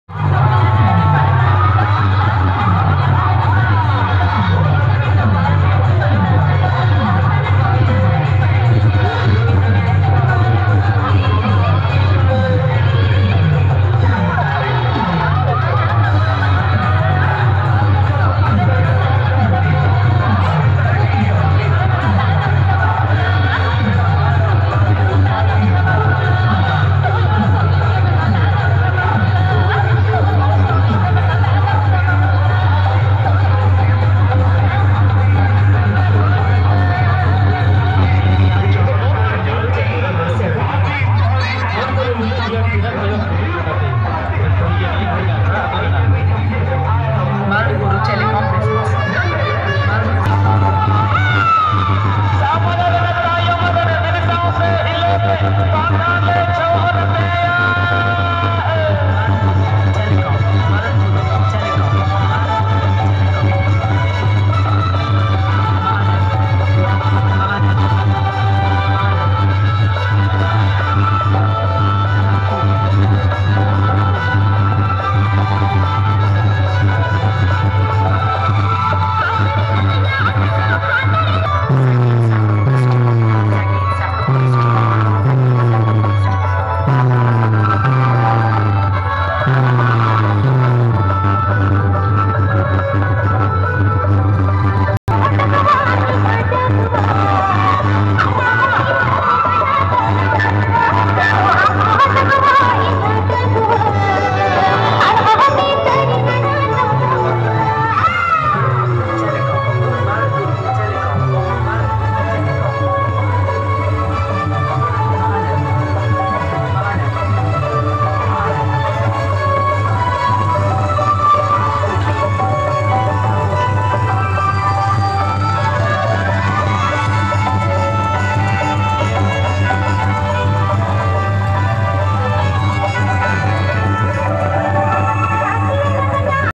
ইলসরা ভাসানে সাউন্ড কিং 👑মেয়ে বৌদি দের কেমন নাচাচ্ছে দেখ। সাউন্ড কিং 👑সিরাজপুর। ভির টা দেখুন
ইলসরা ভাসানে সাউন্ড কিং 👑মেয়ে বৌদি দের কেমন নাচাচ্ছে দেখ। সাউন্ড কিং 👑সিরাজপুর।